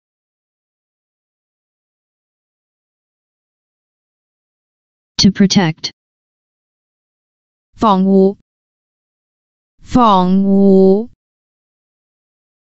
"TO PROTECT" in Cantonese (防護) - Flashcard
[fong4 wu6] "TO PROTECT" in Cantonese (防護) - Flashcard. Additional information about the word: to defend,to protect.